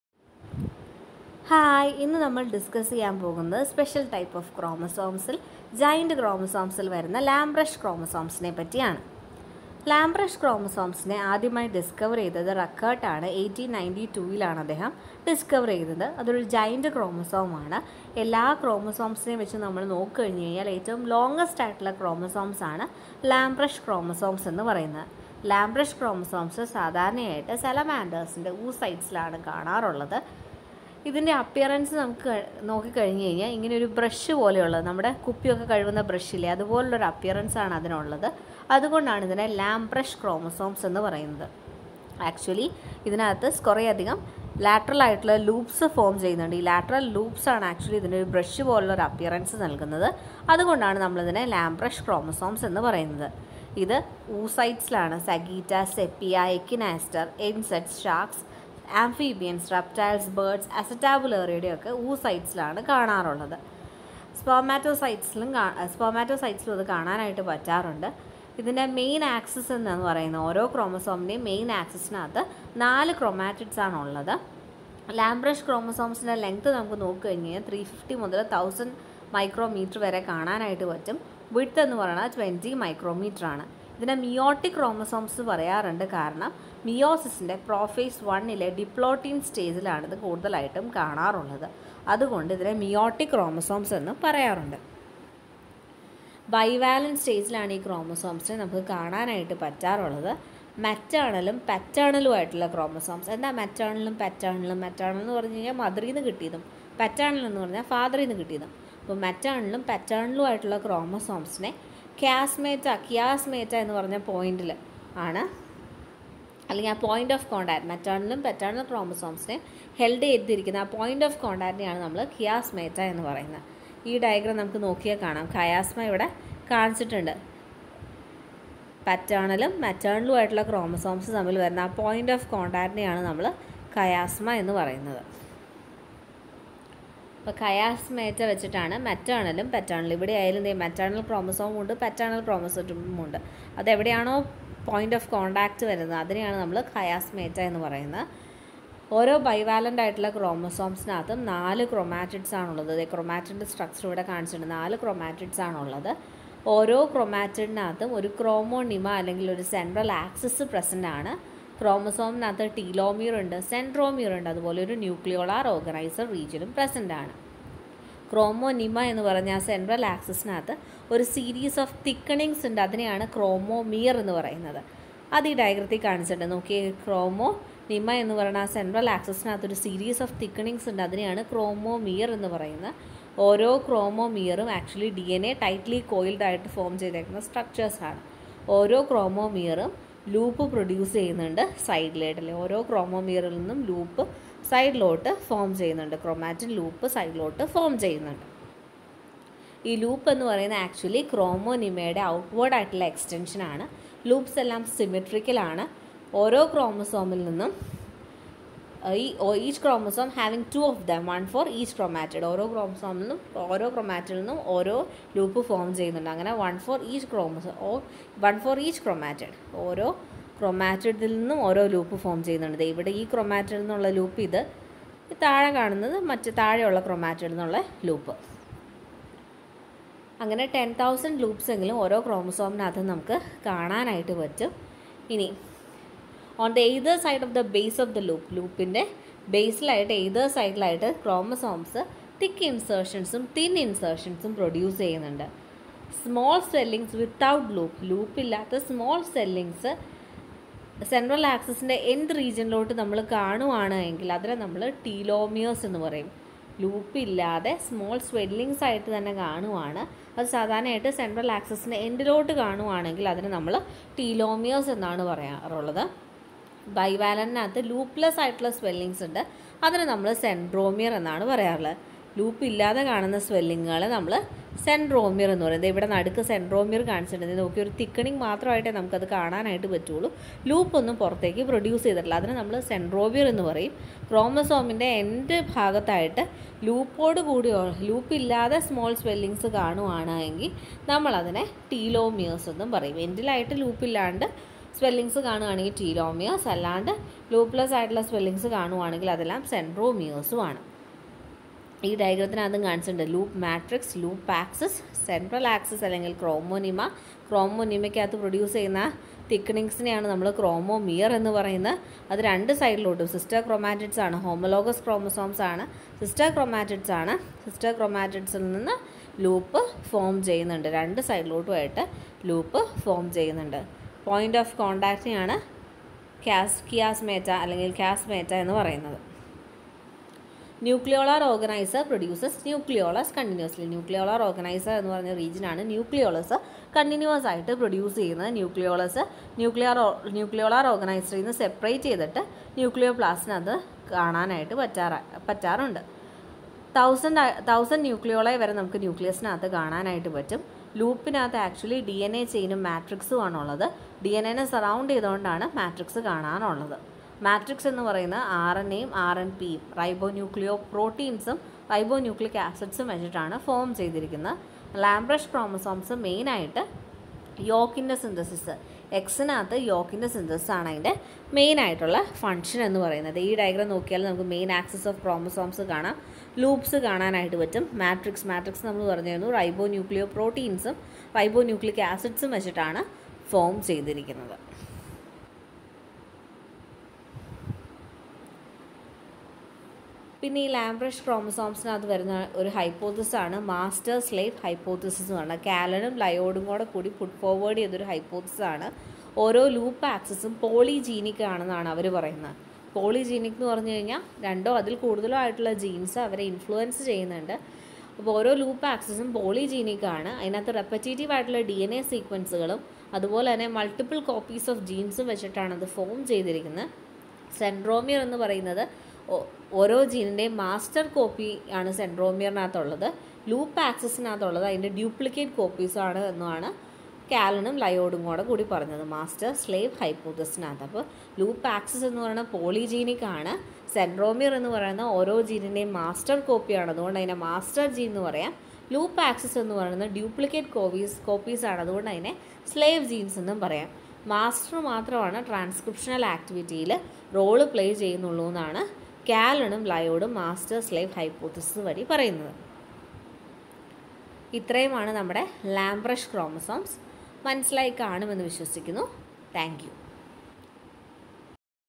Exploring the Unusual: Lampbrush Chromosomes Unveiled! 🧬Malayalam Explanation
Exploring the Unusual: Lampbrush Chromosomes Unveiled! 🧬
🔍 Dive into the extraordinary world of lampbrush chromosomes, a unique and captivating aspect of cell biology. Join us as we unravel the mysteries behind these oversized and elaborately structured chromosomes, prominently found in the oocytes of certain organisms. From their distinctive appearance under the microscope to their crucial role in gene expression, this video provides an in-depth exploration of lampbrush chromosomes. Whether you're a biology enthusiast or simply intrigued by the marvels of cellular life, don't miss this opportunity to discover the specialized and fascinating realm of lampbrush chromosomes. Tune in for an illuminating journey into the intricacies of genetic regulation!